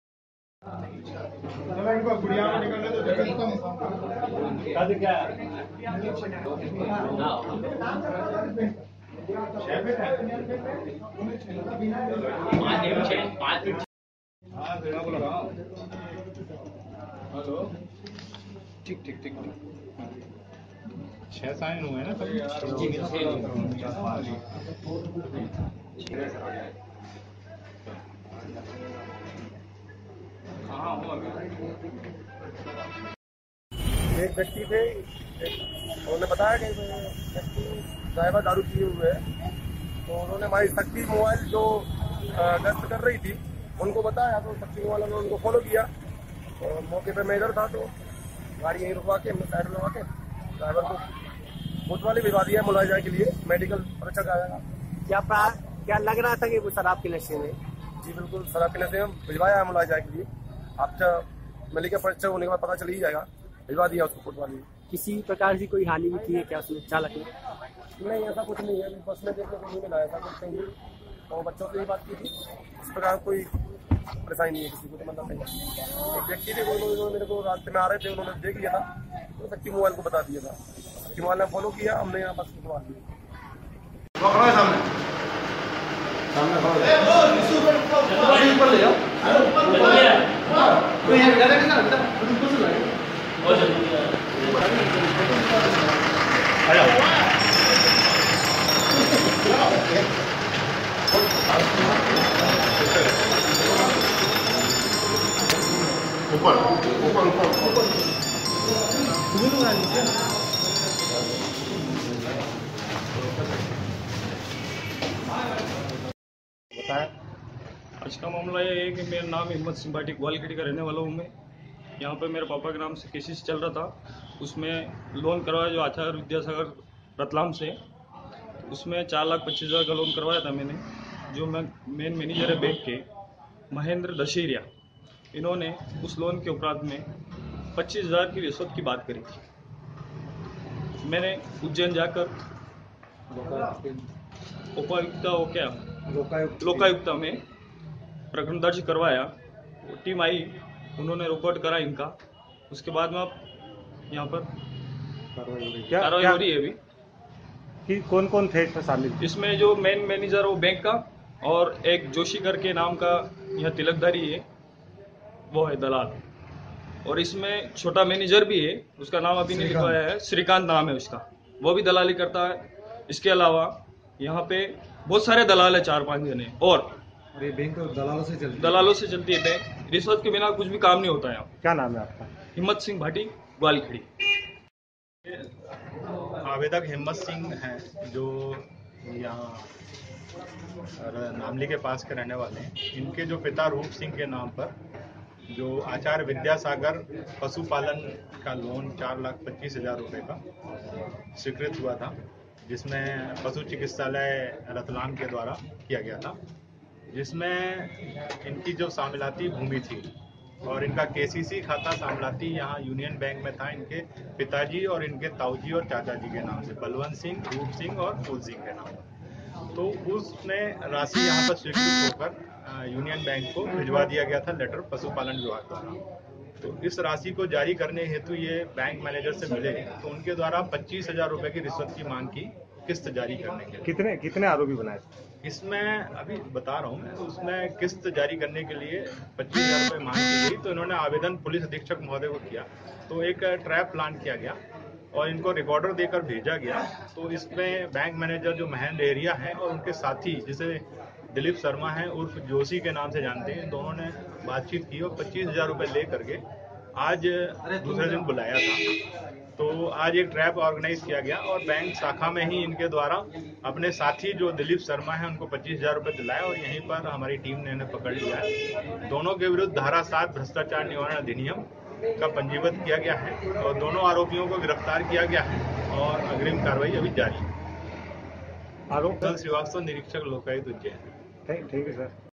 यूनियन बैंक के मैनेजर और दलाल को रिश्वत लेते गिरफ्तार किया
मध्य प्रदेश के रतलाम जिले के नामली में लोकायुक्त पुलिस ने यूनियन बैंक से लोन की किस्त निकालने एवज में 25 हजार रुपए की रिश्वत लेते मैनेजर और उसके दलाल को रंगे हाथों गिरफ्तार किया